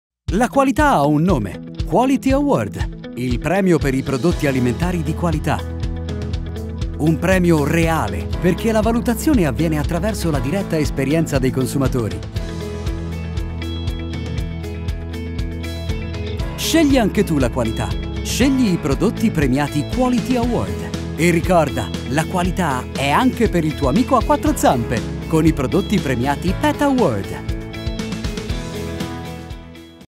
La qualità premiata dai consumatori
Spot Tv relativo ai prodotti alimentari di qualità premiati dai consumatori con il QUALITY AWARD 2018  e ai prodotti per animali premiati con il PET AWARD 2018, on air in TV su FOOD NETWORK , VH1 e RADIO ITALIA.